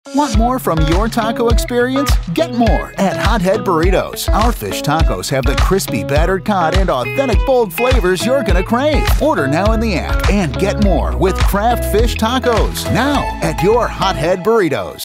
Fish Tacos
Hot Head Fish Tacos are loaded with crispy fish, fresh pico, lettuce, monterey jack cheese with a hint of lime and our famous Hot Head Sauce!

Catch your fish taco today!